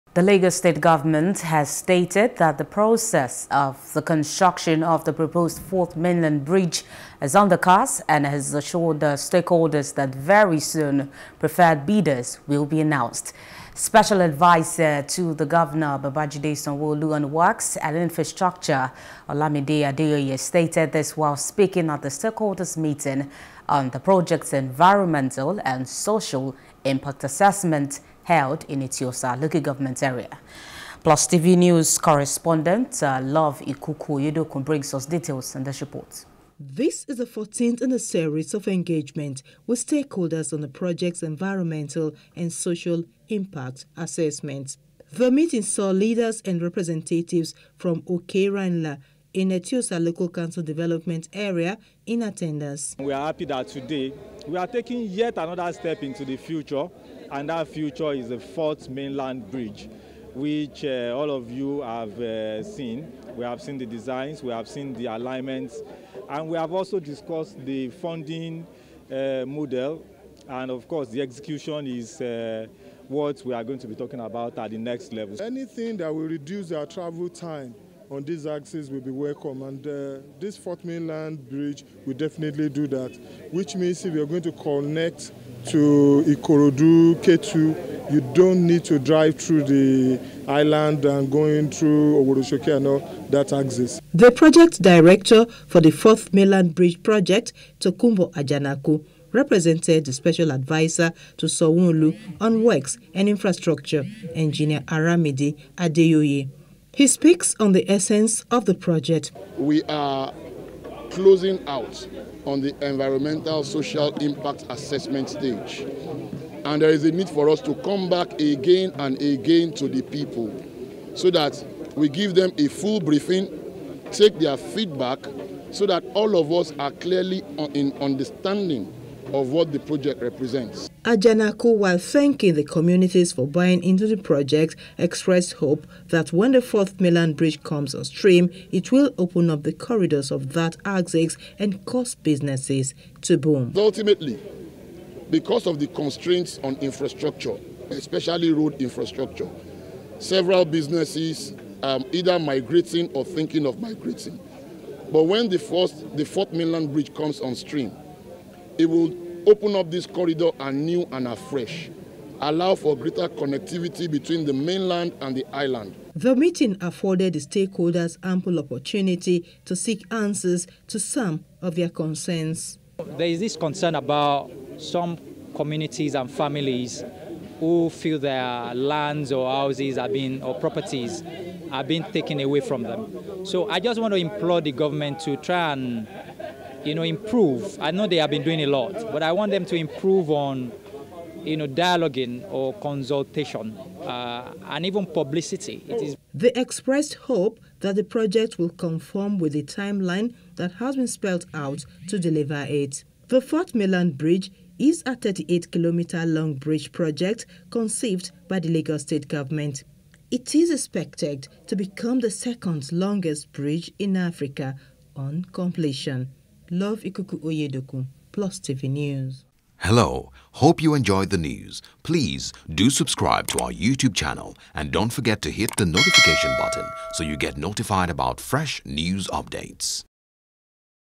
4th Mainland Bridge Construction: Govt. Meets With Stakeholders To Ensure Smooth Take-Off | NEWS
The Lagos State government has stated that the process of the construction of the proposed 4th mainland bridge is on course and has assured stakeholders that very soon, preferred bidders would be announced.